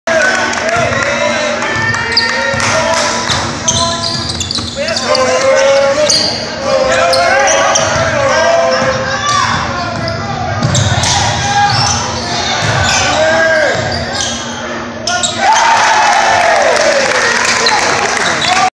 HS Basketball Devon Buckhanon scores 2 of his 5 points. Oak Ridge 86-64 F.
HS Basketball